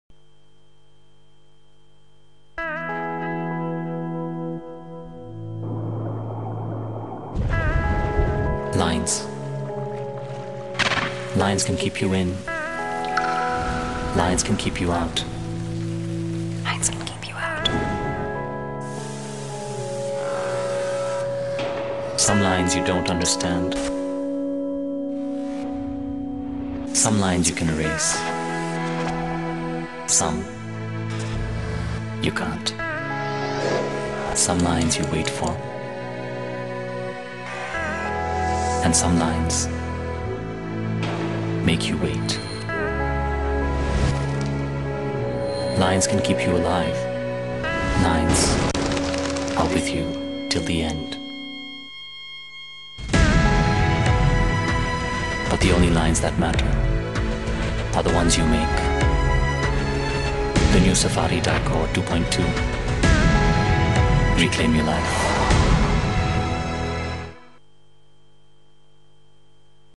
Reclaim your Life - Tata Safari Dicor TV Commercial
Advertisement for Tata Safari Dicor 2.2

Amazingly piercing, metaphorical picturization and concept of the commercial.